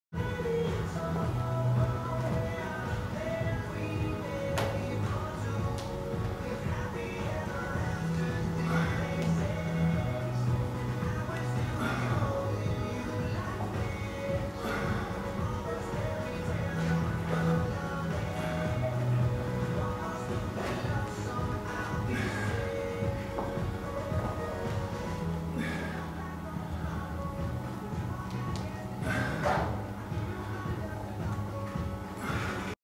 Lunge Matrix
This sequence of exercises is used as part of the BQ Pre-hab routine.